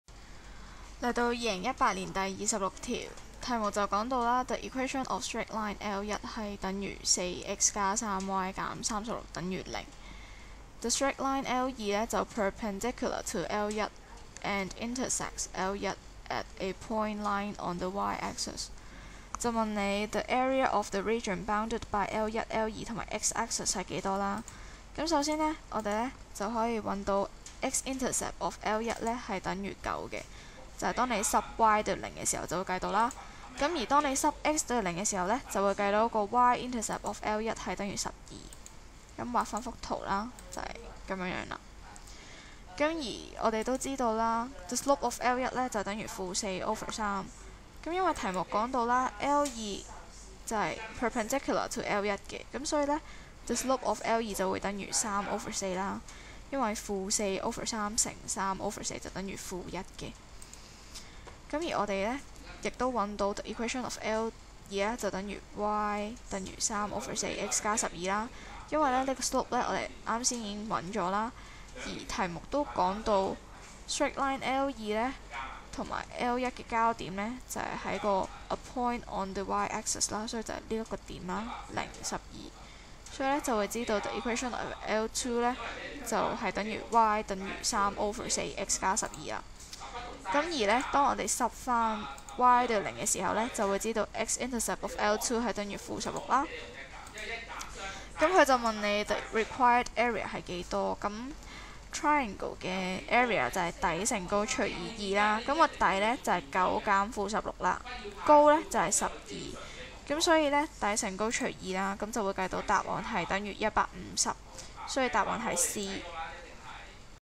2018 HKDSE Maths Core Paper 2 Solution q26, Equation of Straight Lines(直線方程) x Area(面積)
2018 HKDSE Maths Core Paper 2 Solution q26, Equation of Straight Lines(直線方程) x Area(面積)

Dr.K.Liu Maths-Erudition Education 博學習坊

Drkliumaths